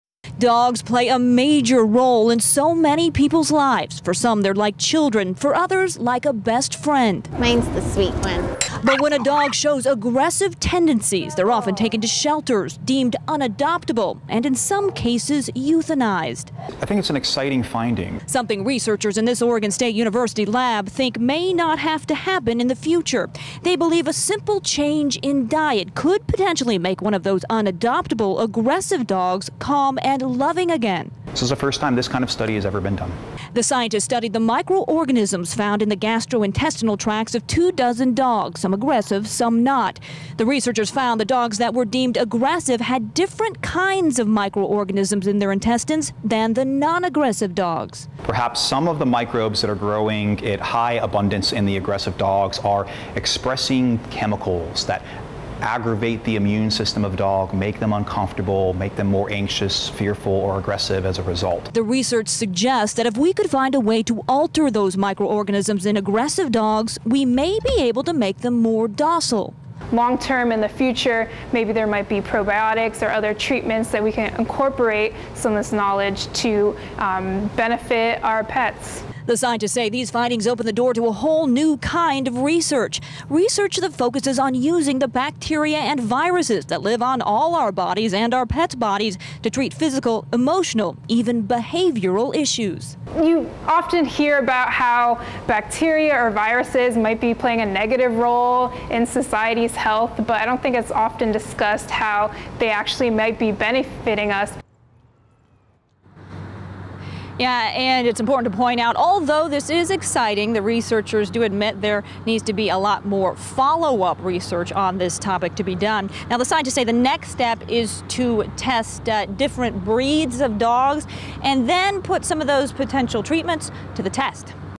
Study links dog bacteria to dog aggression
An OSU study says the bacteria in a dog’s gut can determine how aggressive they are.